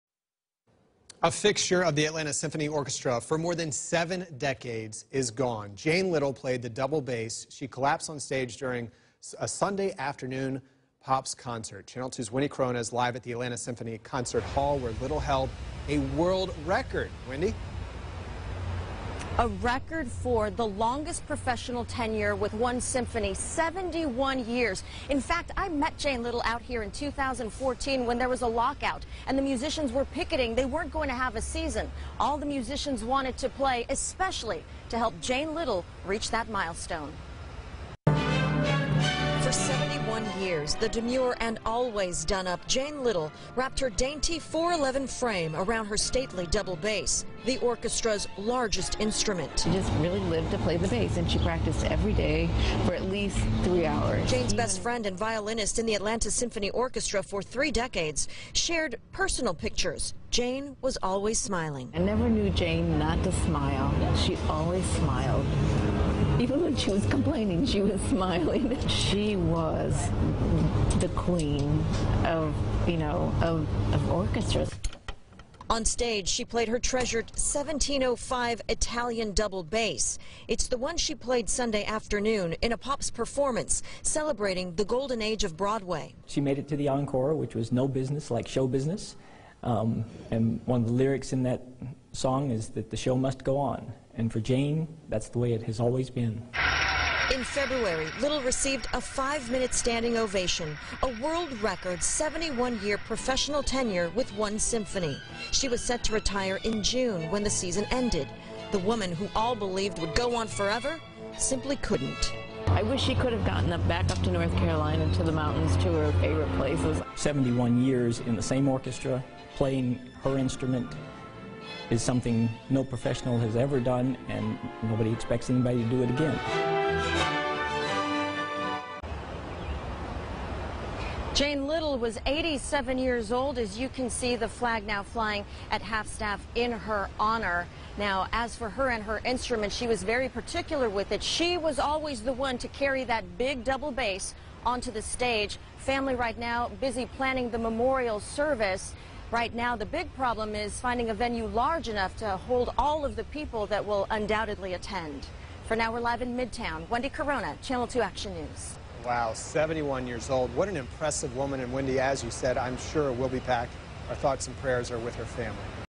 Atlanta Symphony Orchestra bassist Jane Little dies after concert at 87
Little played the double bass for the Atlanta Symphony Orchestra since its founding in 1945, when it was still a youth orchestra.

During an afternoon pops concert encore, Little collapsed on stage. She later died.